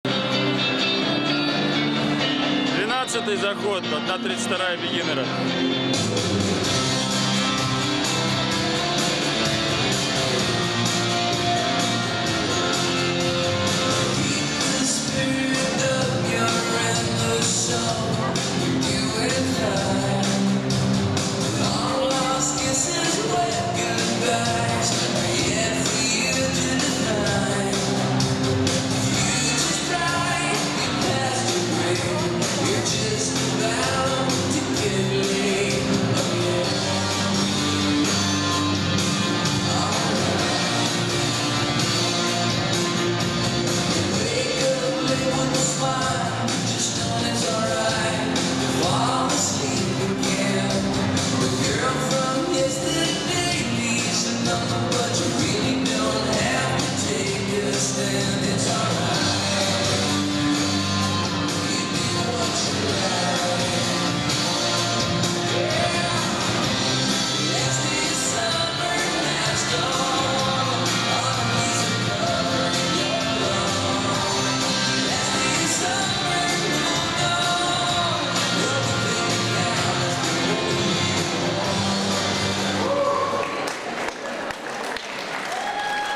ЧМ2017ДнД2-й день J&J Beginner 1/32 заход 12 смена 1
Опубликовано: 15 мая 2017 г.
Чемпионат Москвы по хастлу и дискофоксу
Второй день - 14 мая 2017 г. - турнир J&J (DnD)
Место проведения: Спортивный Комплекс Школы Олимпийского Резерва "ТРИНТА"
Организаторы проведения: Ассоциация Спортивного Хастла
Непосредственные организаторы: Гриненко Никита, Ганеева Эльвира и Ко
видеооператор: Игорь Скобелев